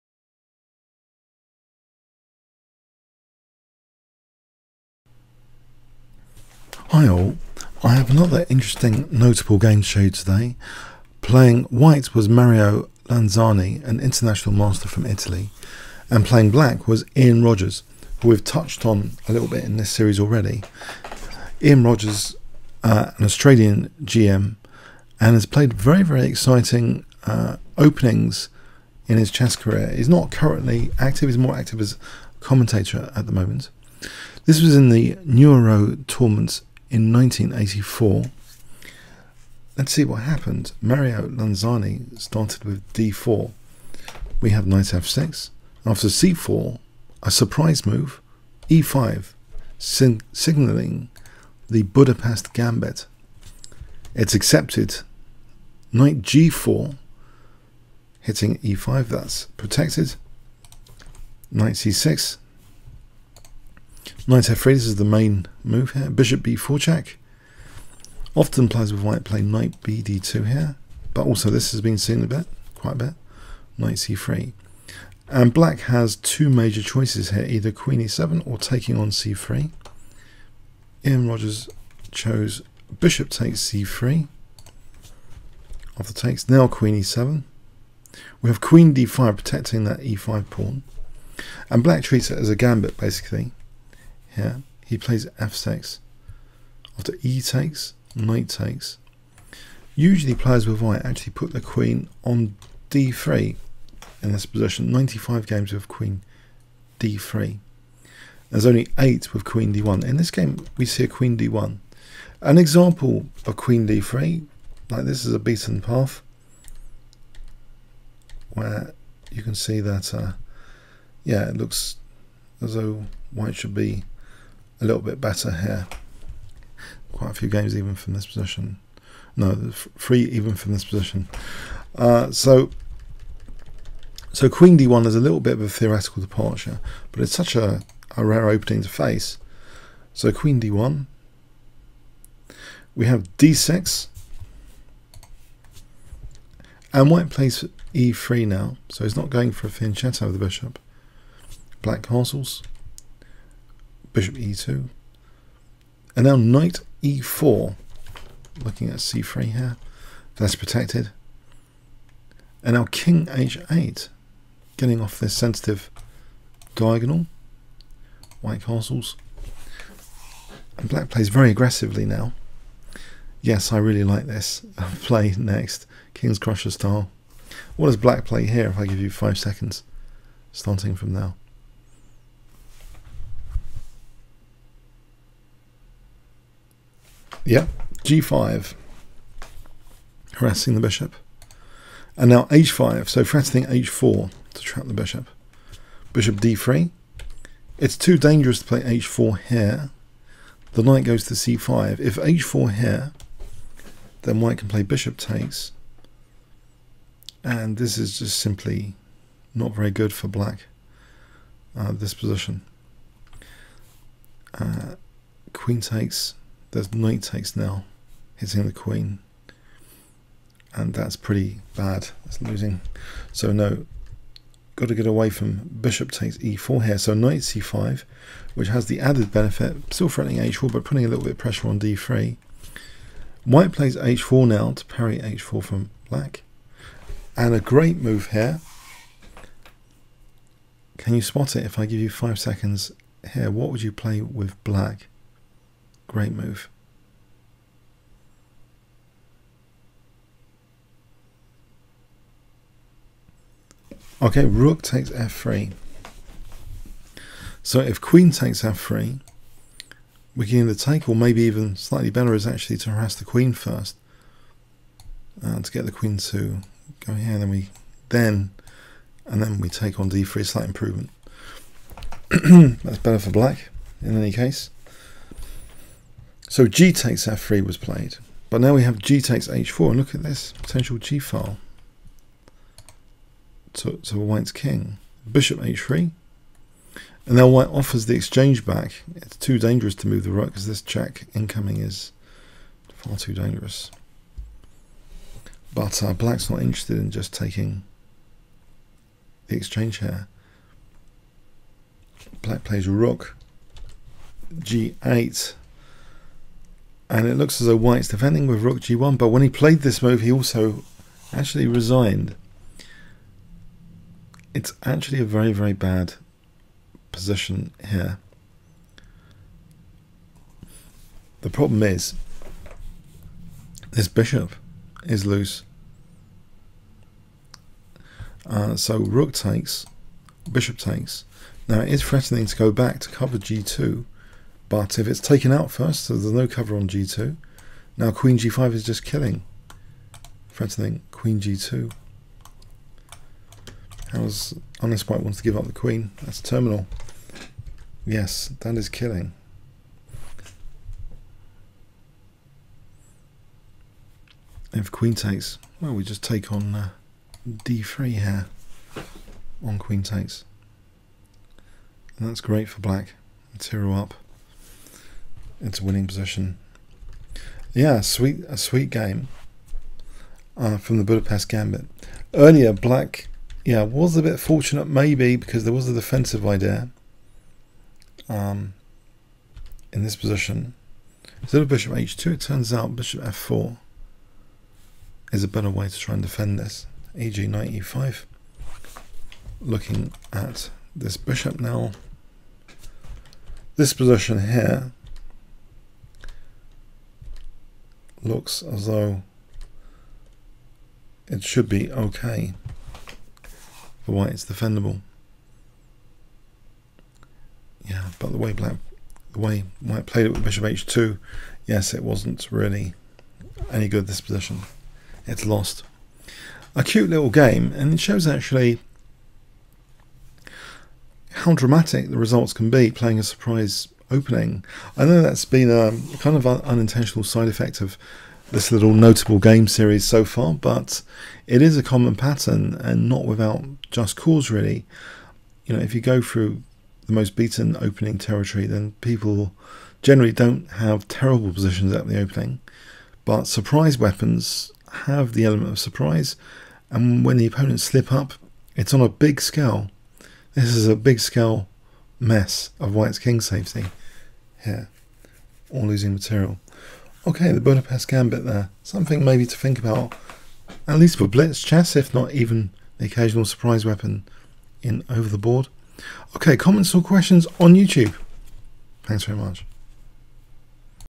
GM Ian Rogers notable chess game: vs Mario Lanzani : Budapest Gambit
[Event "Nuoro"]
[Site "Nuoro"]
[Date "1984.??.??"]
[EventDate "?"]
[Round "?"]
[Result "0-1"]
[White "Mario Lanzani"]
[Black "Ian Rogers"]
[ECO "A52"]
[WhiteElo "?"]
[BlackElo "?"]
[PlyCount "41"]

1.d4 Nf6 2.c4 e5 3.dxe5 Ng4 4.Bf4 Nc6 5.Nf3 Bb4+ 6.Nc3 Bxc3+
7.bxc3 Qe7 8.Qd5 f6 9.exf6 Nxf6 10.Qd1 d6 11.e3 O-O 12.Be2 Ne4
13.Rc1 Kh8 14.O-O g5 15.Bg3 h5 16.Bd3 Nc5 17.h4 Rxf3 18.gxf3
gxh4 19.Bh2 Bh3 20.Kh1 Rg8 21.Rg1 0-1